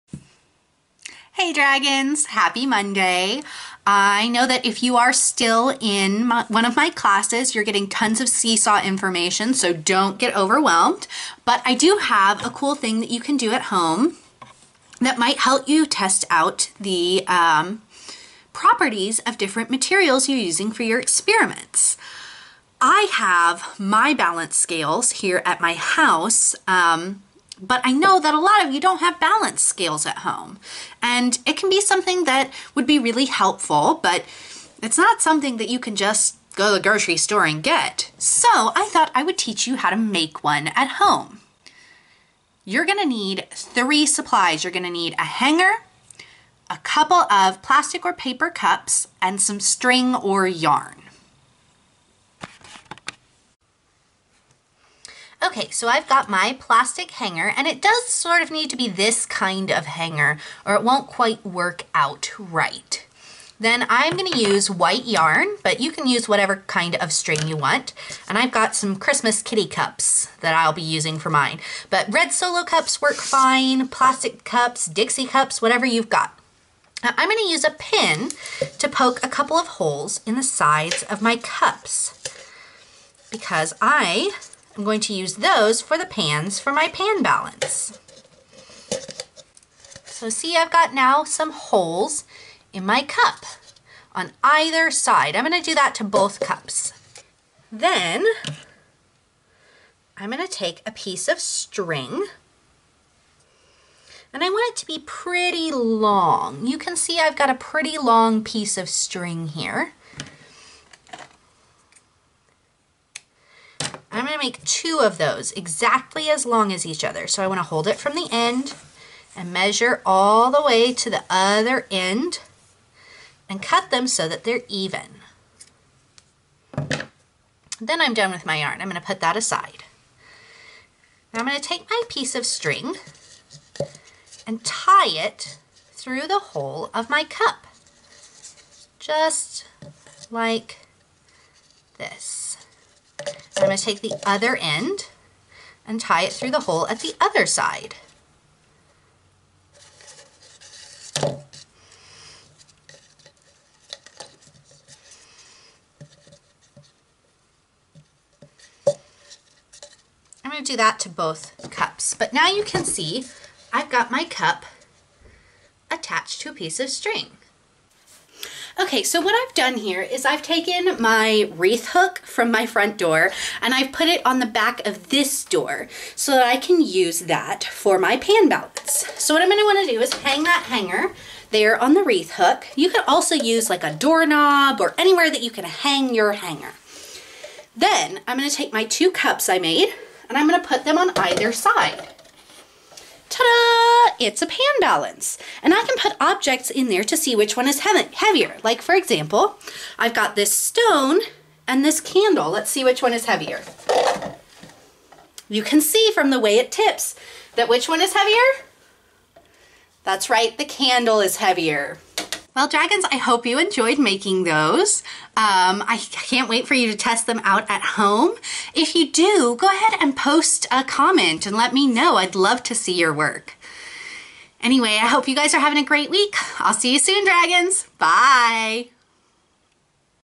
Pan Balance!
Learn how to make a pan balance at home so you can test the properties of the materials you're using!